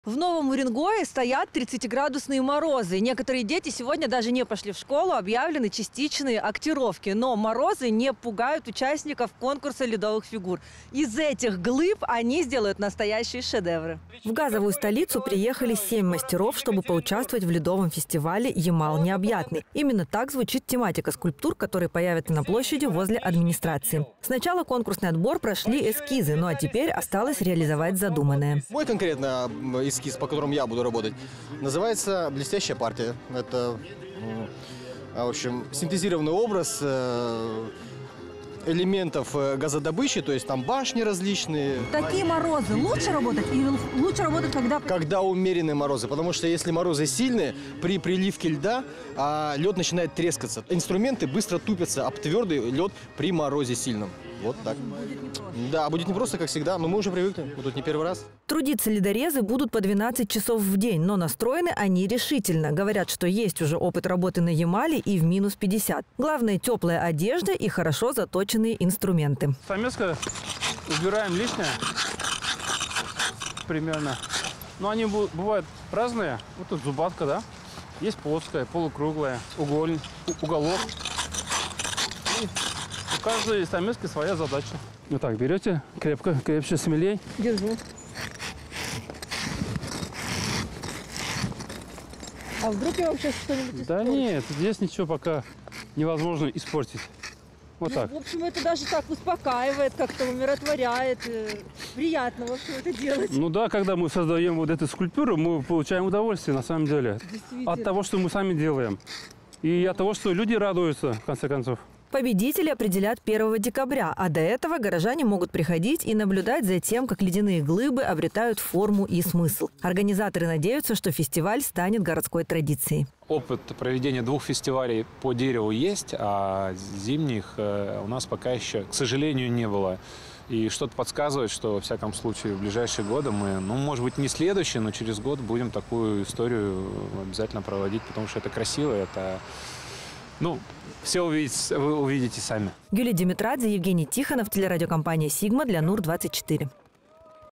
ФЕСТИВАЛЬ «НЕОБЪЯТНЫЙ ЯМАЛ»
В НОВОМ УРЕНГОЕ ОТКРЫЛСЯ ФЕСТИВАЛЬ ЛЕДОВЫХ СКУЛЬПТУР